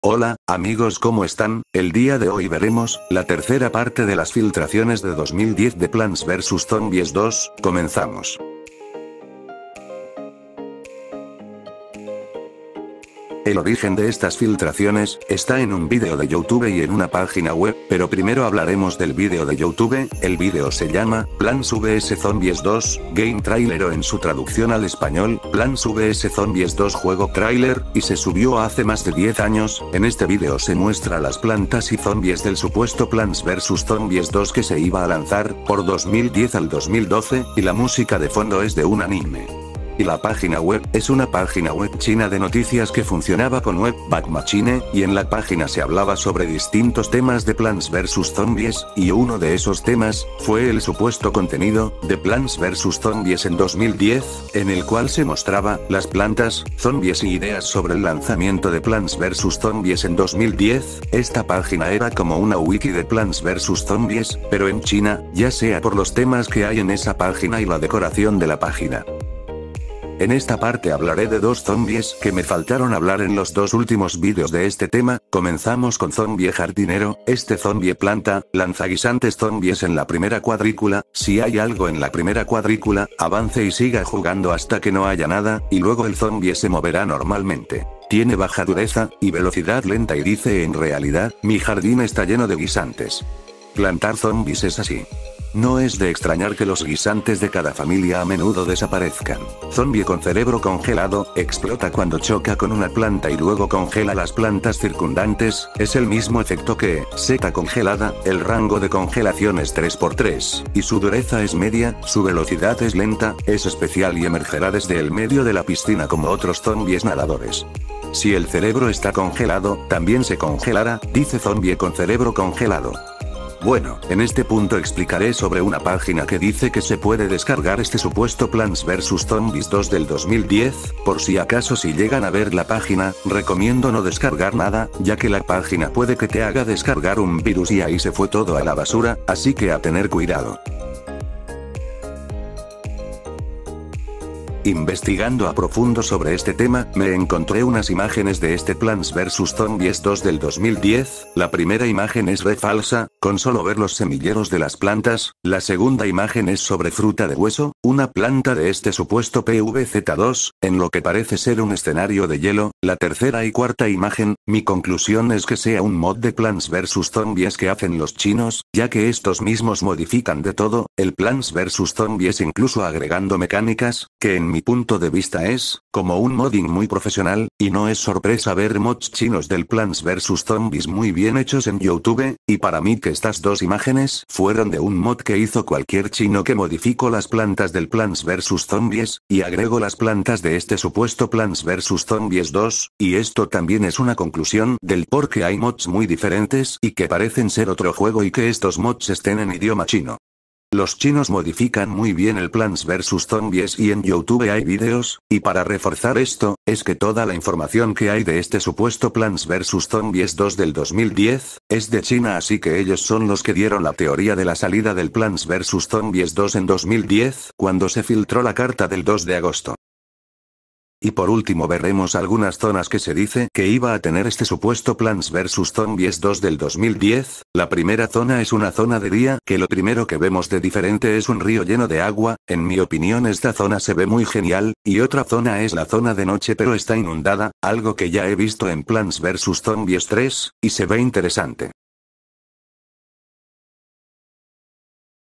Las Supuestas Filtraciones de Plants Vs Zombies 2 en 2010-2012 - Parte 3
Capítulos:
00:00 Inicio
00:11 Origen
01:09 Zombies
02:02 Página falsa de descarga
02:22 Imágenes del supuesto pvz2 del 2010
03:40 Zonas-Escenarios